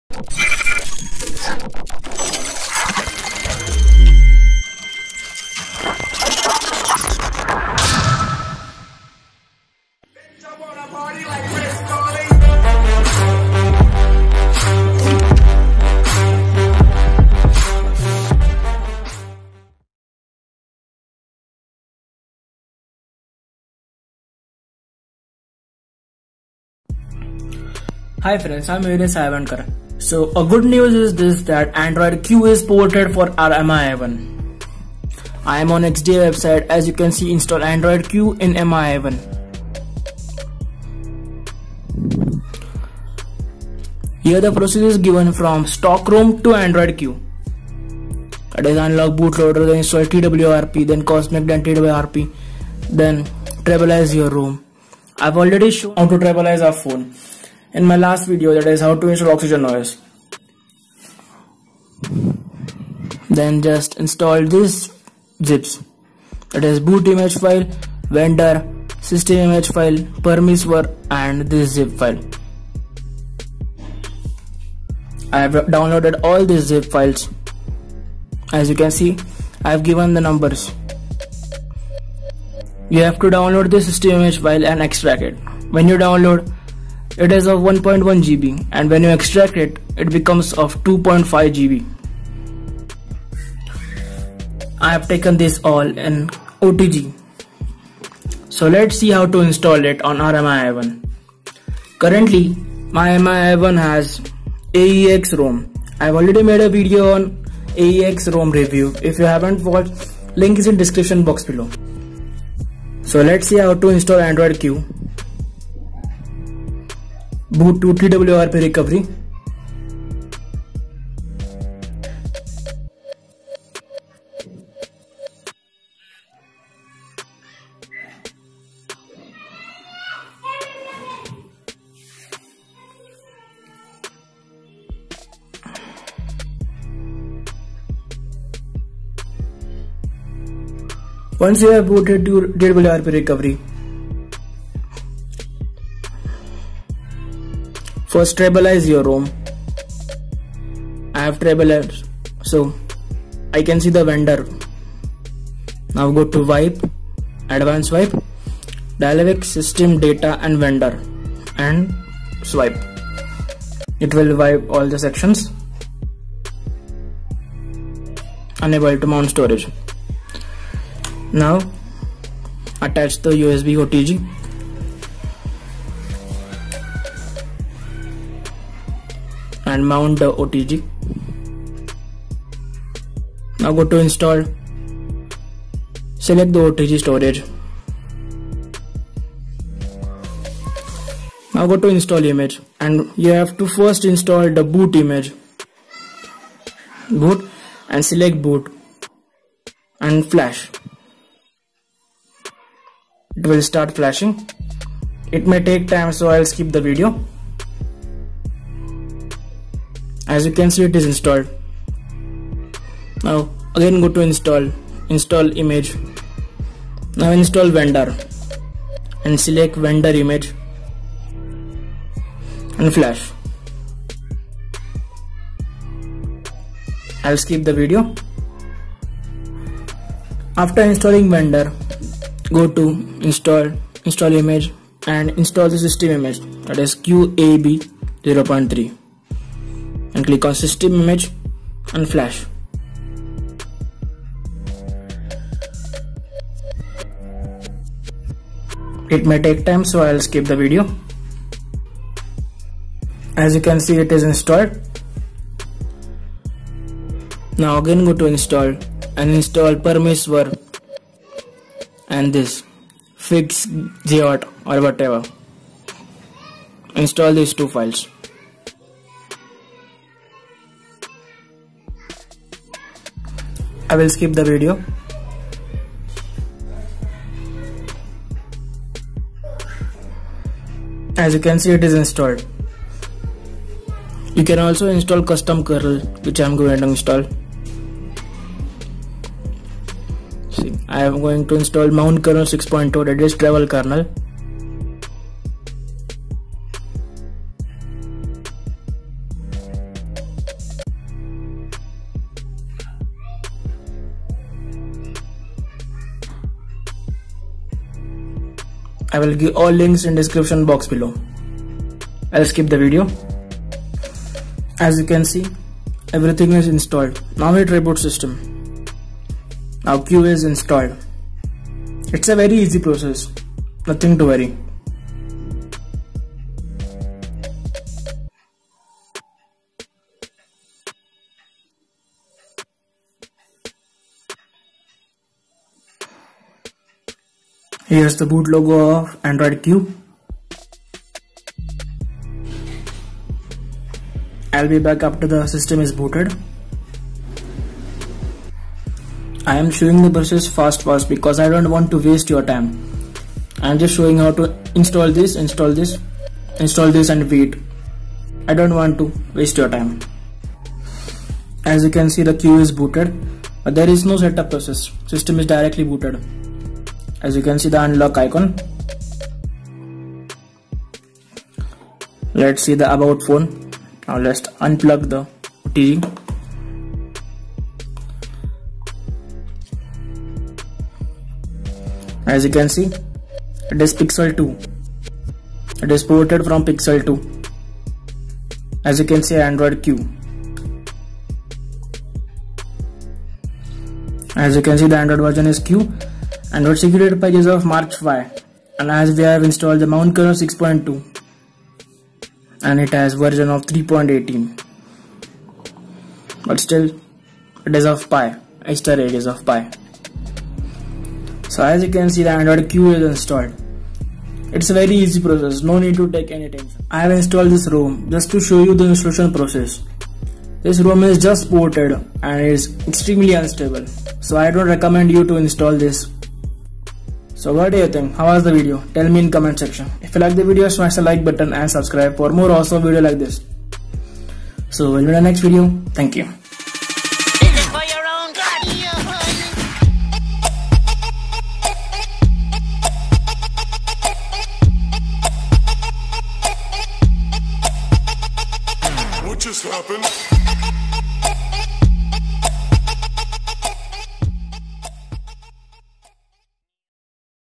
How to install Android Q beta on Mi A1
Guys in this video I have shown you How to install Android Q on Mi A1 if you like the video smash the like button and subscribe for more awesome content like this.

Want to Join My YouTube family  ❤